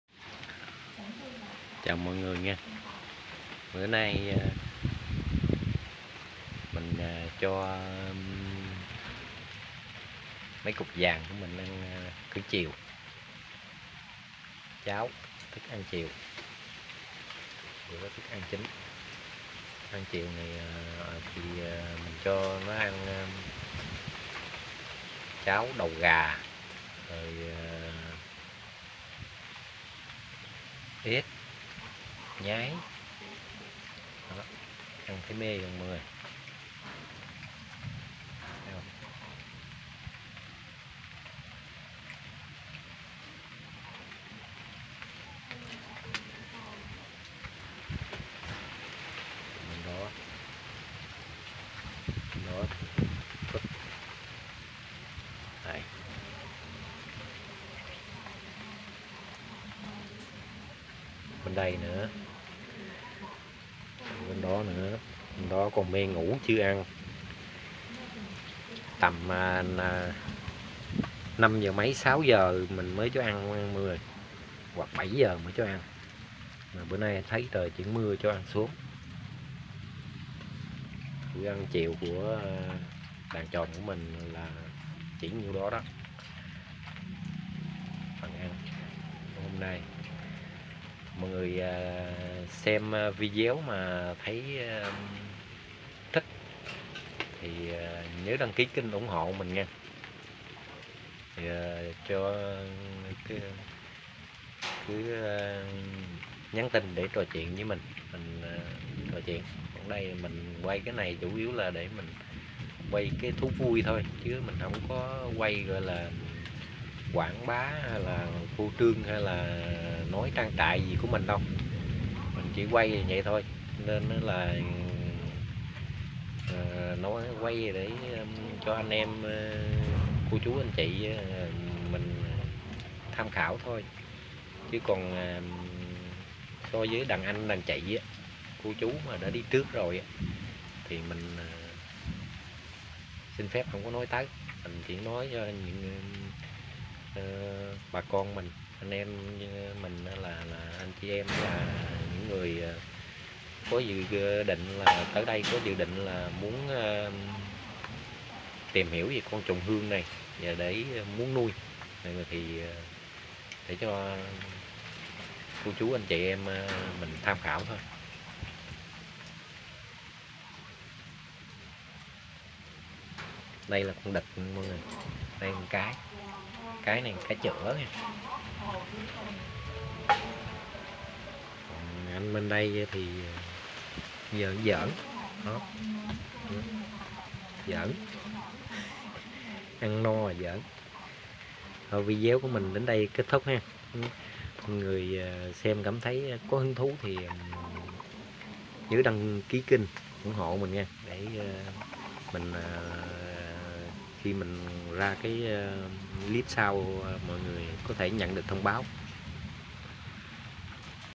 bữa ăn chính của thú cưng mình nha..0379272652
mô hình chồn hương thuần của mình chỉ đang tăng đan lên nha mọi người . mình chỉ mới nui thôi nên chưa gây dan nhui . mọi người có hứng thú thì co men cho mình hoặc liên hệ với mình qua số điện thoại.0379272652 để trò chuyện nha